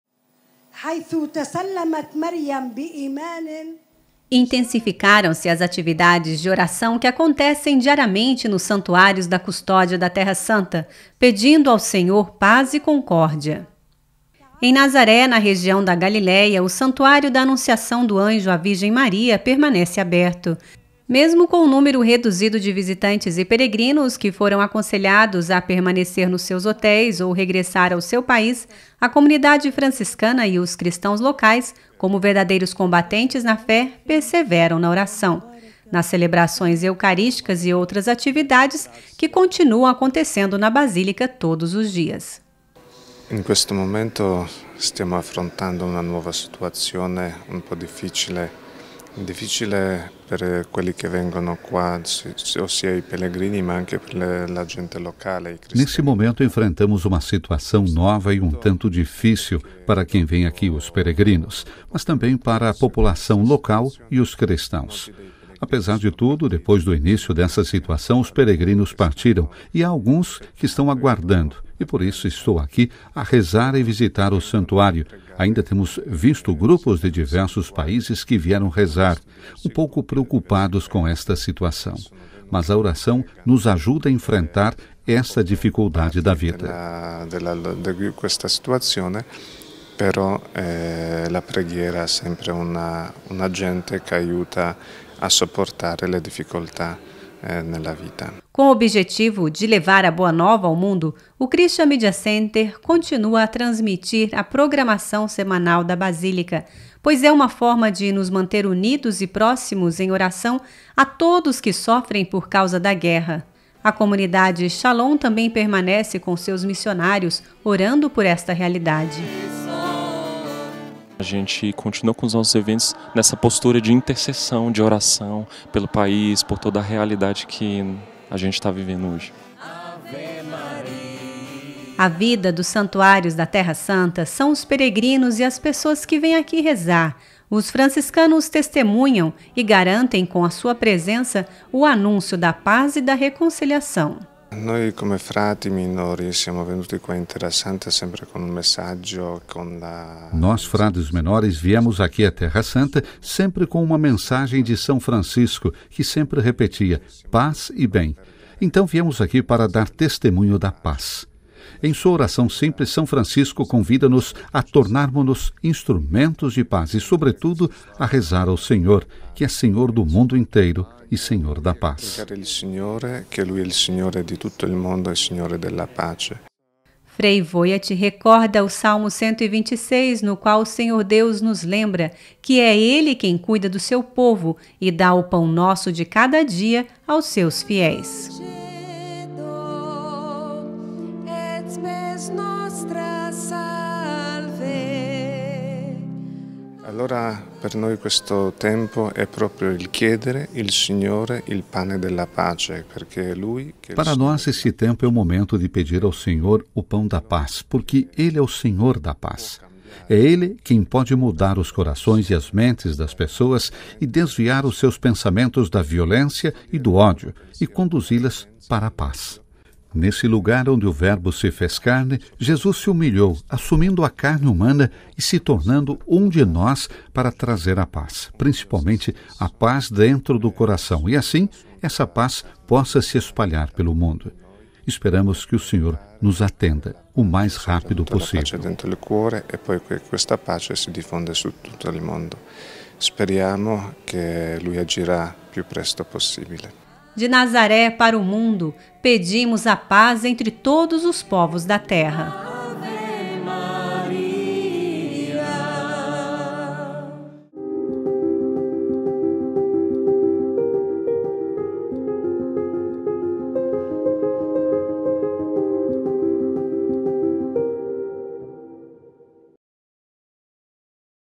Do Santuário de Nazaré, um apelo pela paz!
Intensificaram-se as atividades de oração que acontecem diariamente nos santuários da Custódia da Terra Santa, pedindo ao Senhor paz e concórdia.

Em Nazaré, na região da Galileia, o Santuário da Anunciação do Anjo à Virgem Maria permanece aberto. Mesmo com um número reduzido de visitantes e peregrinos, que foram aconselhados a permanecer nos seus hotéis ou regressar ao seu país, a comunidade franciscana e os cristãos locais, como verdadeiros combatentes na fé, perseveram na oração, nas celebrações eucarísticas e outras atividades que continuam acontecendo na Basílica todos os dias.